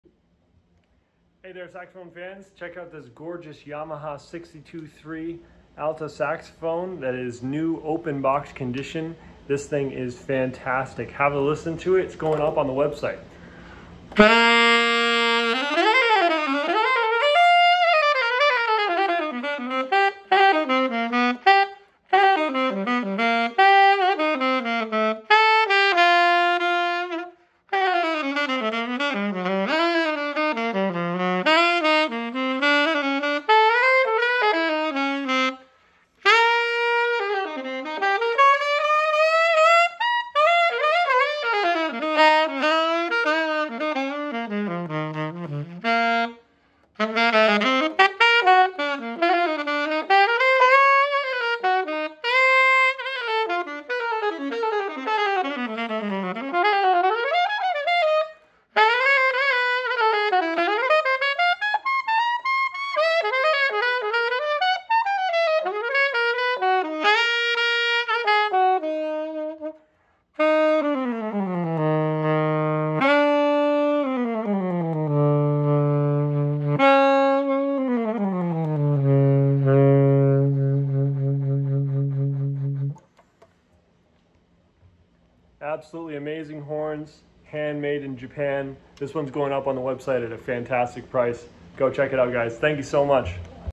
Play Testing This Beautiful Yamaha YAS 62III Silver Alto Saxophone!
Sax Fans! Check out this GORGEOUS and Rarely in stock Yamaha 62III Professional Alto in Silver.🔥🔥🔥

The newly redesigned Yamaha YAS-62 III Alto Sax in silver finish. This professional saxophone has long been a favorite for advancing players. It plays with a fast, easy sound, and good control. It features keywork to high F#, and a newly designed neck. The new 62 neck has a slightly narrower bore for a faster response and greater control. Comes in a backpack style case.